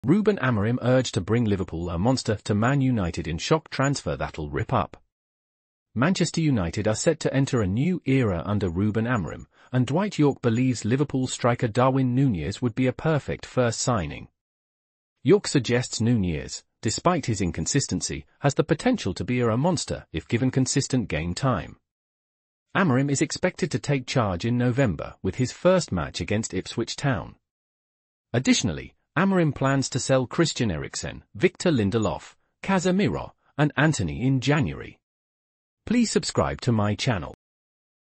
Ruben Amorim urged to bring Liverpool ‘monster’ to Man Utd in shock transfer that’ll ‘rip up’…
Manchester United are set to enter a new era under Ruben Amorim, and Dwight Yorke believes Liverpool striker Darwin Nunez would be a perfect first signing. Yorke suggests Nunez, despite his inconsistency, has the potential to be a 'monster' if given consistent game time. Amorim is expected to take charge in November, with his first match against Ipswich Town. Additionally, Amorim plans to sell Christian Eriksen, Victor Lindelof, Casemiro, and Antony in January.